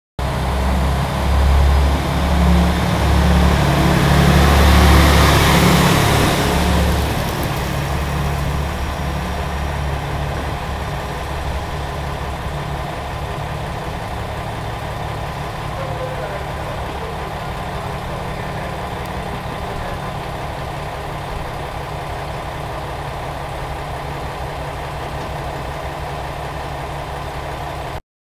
【三次駅】キハ120 発車 【HD】2013.10.05
三次駅にて
キハ120の発車です。